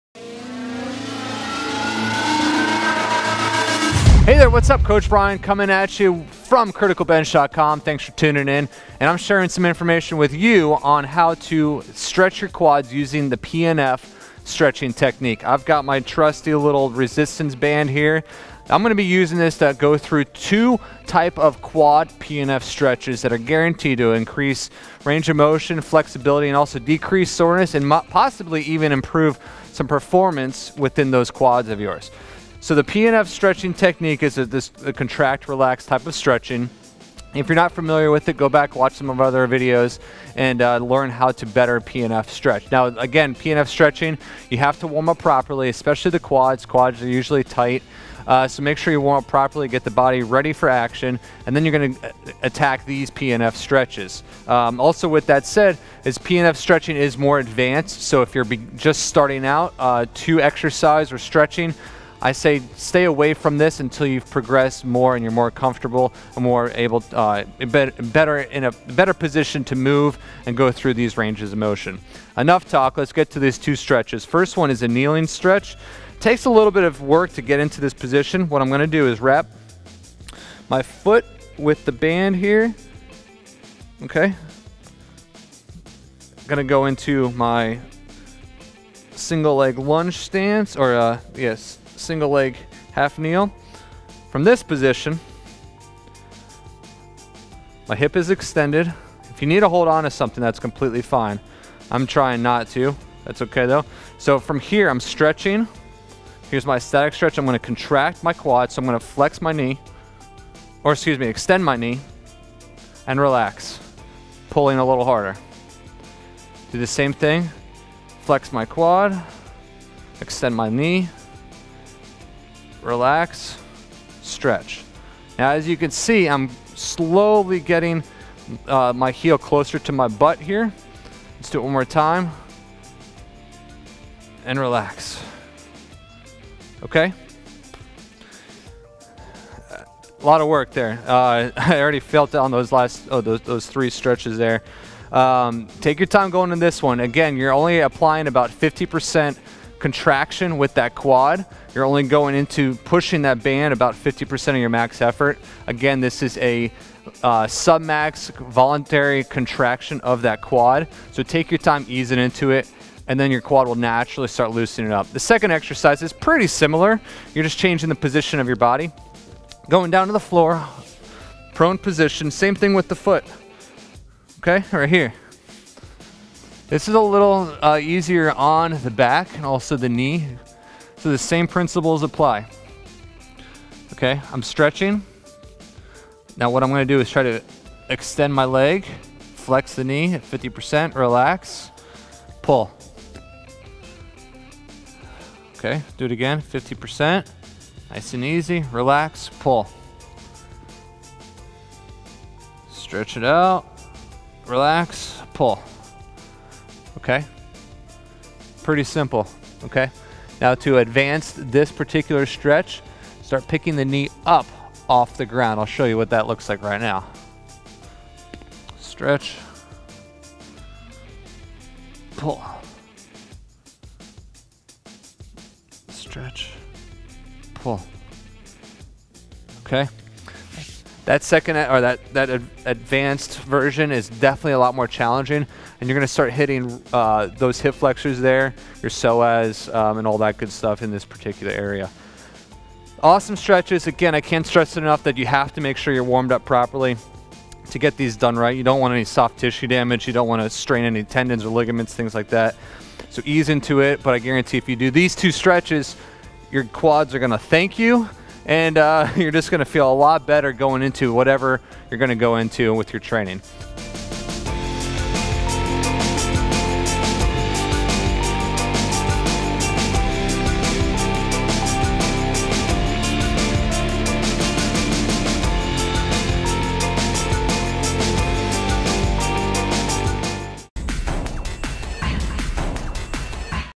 Two PNF Stretches for your QUADS
Why Stretching Won't Make You Flexible - FREE report

In this video, Strength & Conditioning Coach Brian Klepacki, shares two PNF quad stretches to decrease soreness and increase range of motion! Follow this tutorial to improve performance within your quads.